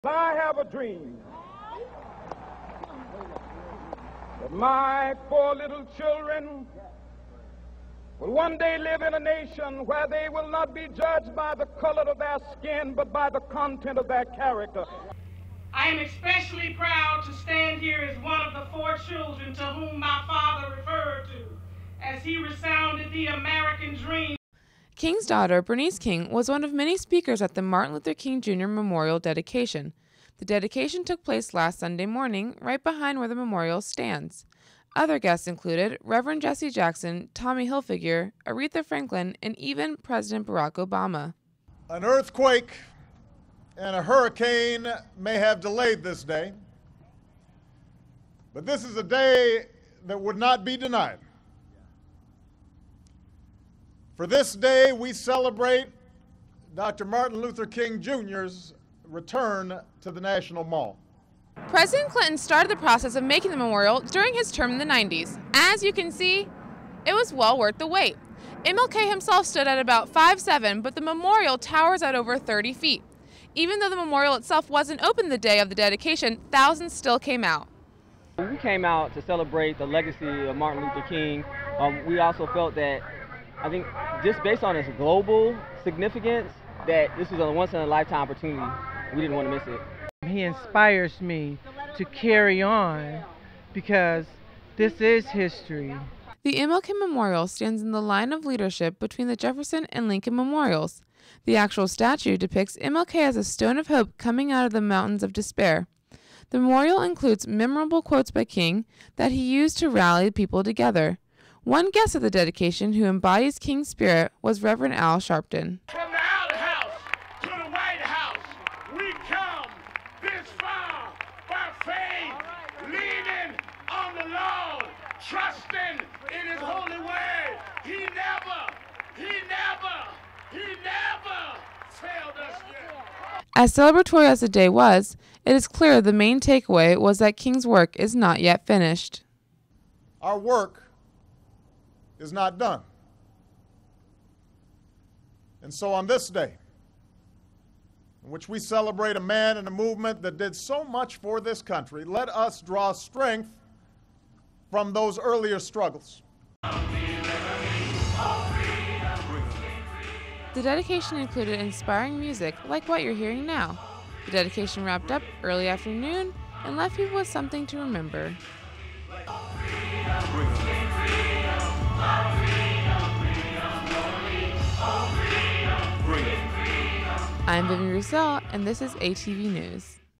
MLK Dedication Ceremony
Monika Fidler reports on the relocation of the Washington College of Law at American University.